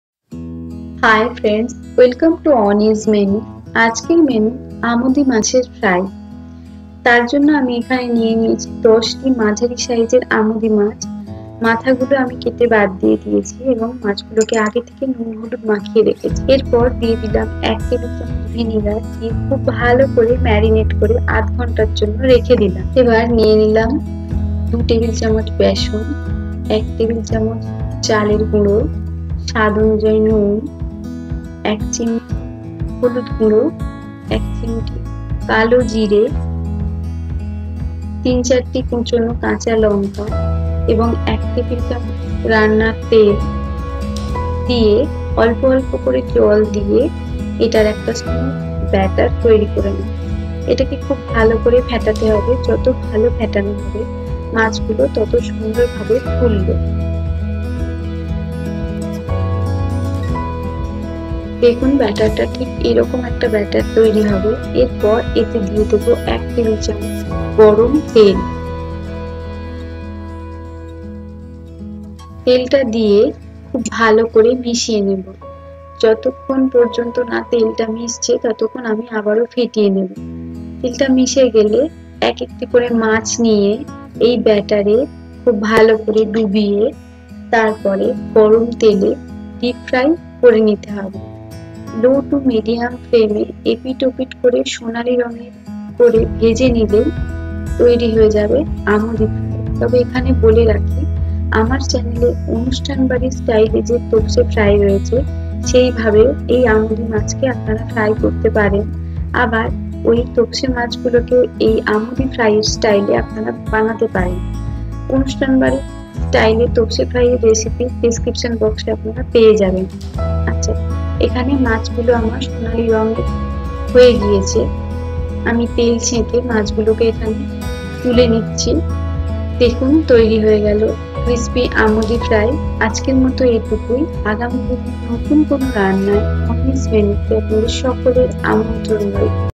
Amudi Fish Fry / Crispy Fish Fry / Fish snacks
Amudi Fish Fry

Ingredients :
Amudi Fish - 10 pcs

Batter preparation :
Gram Flour / Besan - 2 tbs
Pinch of black cumin / Kala jeera
Water required to prepare smooth batter
Warm oil - 1 tbs

Oil required for dipped fry